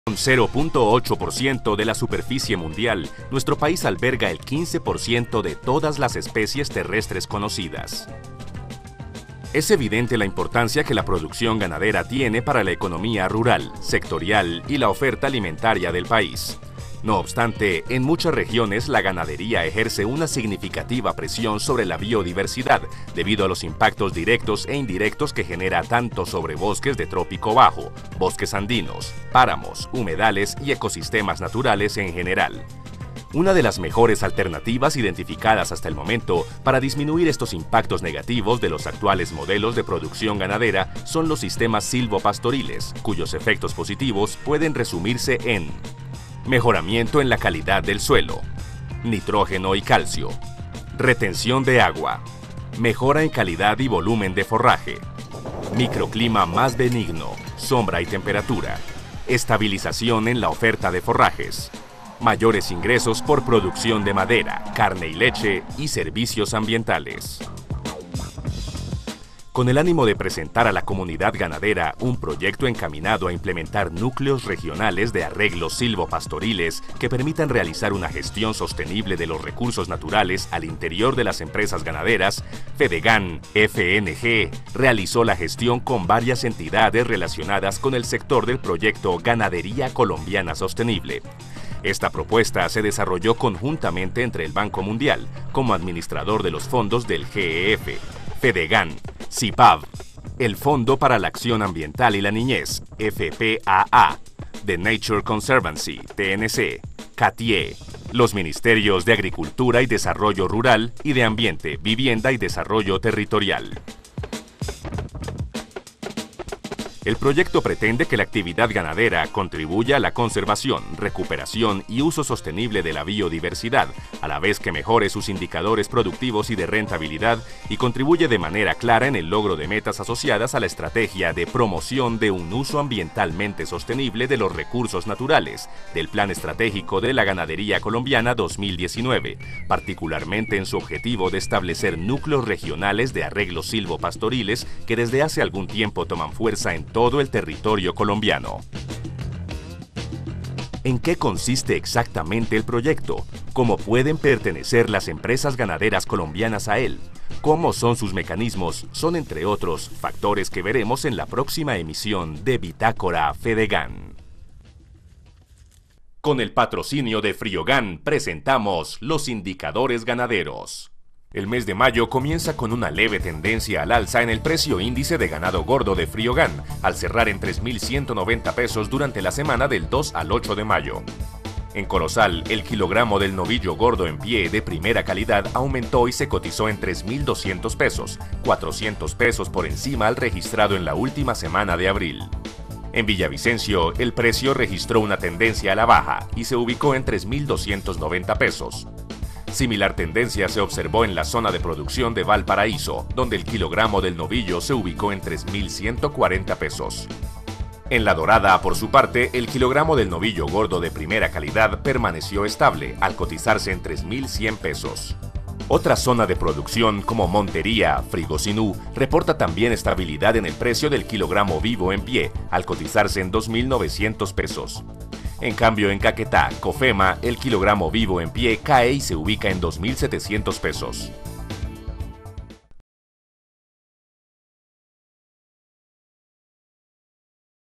Producción Ganadera en la Economía Rural - TvAgro por Juan Gonzalo Angel
La ganadería es una actividad económica de origen muy antiguo que consiste en el manejo y explotación de animales domesticables con fines de producción, para su aprovechamiento (véase producción de leche, avicultura, porcicultura). En cambio, el manejo de animales pertenecientes a especies silvestres (no domésticas) en cautiverio o en semicautiverio se conoce con el nombre de zoocría.

Dependiendo de la especie ganadera, se pueden obtener diversos productos derivados, tales como la carne, la leche, los huevos, los cueros, la lana y la miel, entre otros.1​ La ciencia encargada del estudio de la ganadería es la zootecnia y los profesionales encargados directamente del desarrollo de la producción animal son los ganaderos, ayudados por los zootecnistas y los ingenieros de producción animal, en estrecha colaboración con los médicos veterinarios que son los encargados de la prevención y control de las enfermedades de los animales.

Los ganados más importantes en número a nivel mundial son los relacionados con la ganadería bovina, la ovina y la porcina. Sin embargo, en algunas regiones del planeta otros tipos de ganado tienen mayor importancia, como el caprino y el equino, como así también la cunicultura, la avicultura y la apicultura.1​

La ganadería está relacionada con la agricultura, ya que en una granja ambas pueden estar relacionadas. En estos casos el ganado aporta el estiércol, que es utilizado como abono, y los cultivos aportan el alimento para los animales.1​